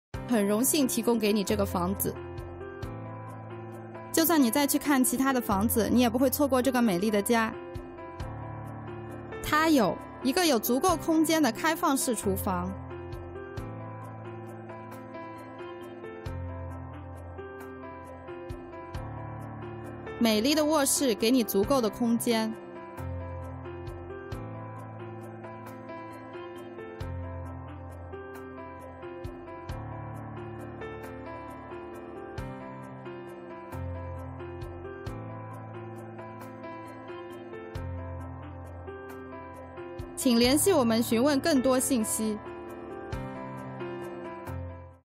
For Sale 30 Elata Street Oakleigh South Vic 3167 - Chinese
For Sale -  30 Elata Street Oakleigh South Vic 3167 Buxton Oakleigh 2 Bedrooms 1 Bathroom 1Garage   A Private Domain For Easy-Care Living Be captivated by the charm, impressed by the comfort and inspired by the convenience of this delightful 2 bedroom, 1 bathroom home. With its private fenced frontage and exemplifying brilliant low-maintenance living, engaging personality suggests exciting options to put some creative flair and contemporary style into the presently immaculate layout. Abundant natural light creates a superb introduction to a spacious lounge merging seamlessly with a pristine kitchen/meals area, enhancing a golden opportunity to break into the property scene, reap some solid investment rewards, downsize with ease or just enjoy a stress-free lifestyle. Featuring a private courtyard, gas heating, evap AC and SLUG, a walk to Huntingdale station and moments to Oakleigh central, Oakleigh South Primary and Sacred Heart.